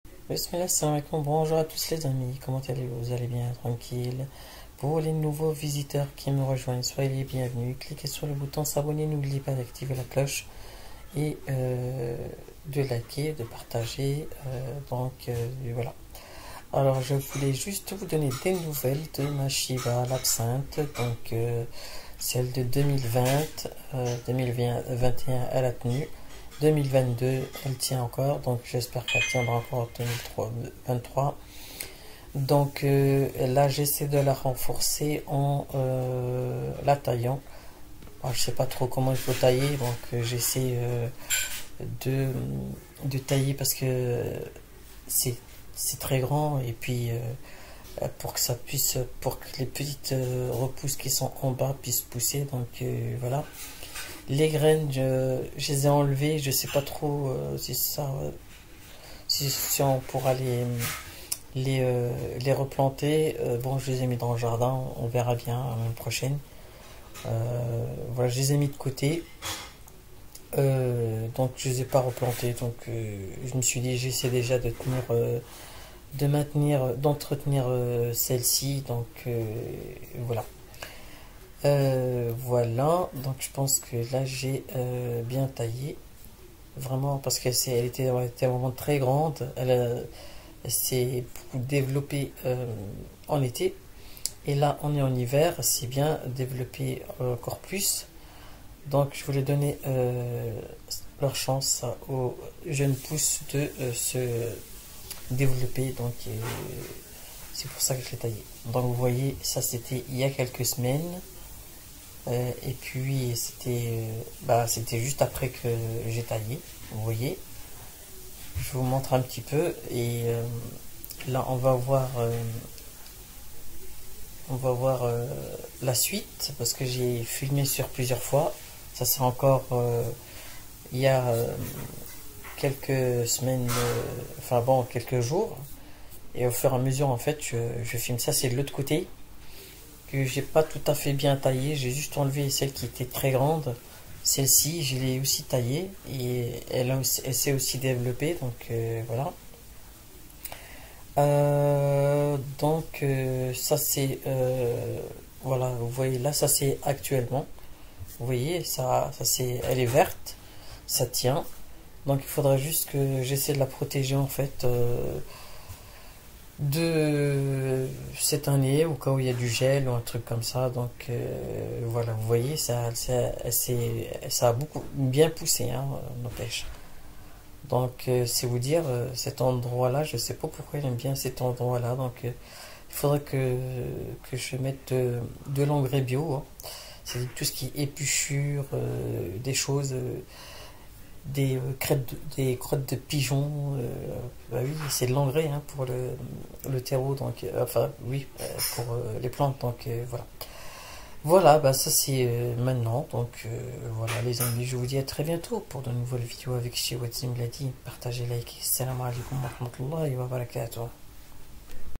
Comment tailler Chiba (Absinthe) en Octobre 2022?
Des nouvelles de ma Chiba !
Elle va bien et en forme machaallah !